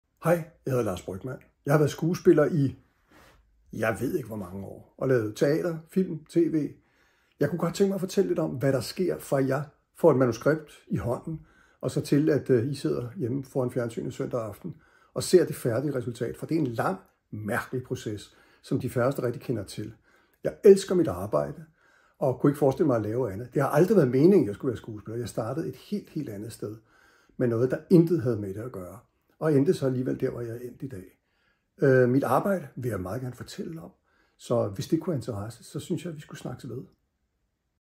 Lars Brygmann Et møde med Lars Brygmann video
Det stod ikke skrevet i stjernerne, at Lars Brygmann skulle være skuespiller. Tilfældigheder gjorde, at den uddannede tømrer kastede sig over skuespillet, og han har gennem årerne markeret sig som en usædvanlig dygtig karakterskuespiller på teater, film og i TV serier.

I dette nye foredrag fortæller Lars Brygmann med humor og nærvær om sit eget liv. Han deler sine livserfaringer med publikum og fortæller om arbejdsglæde, livet og alt midt i mellem.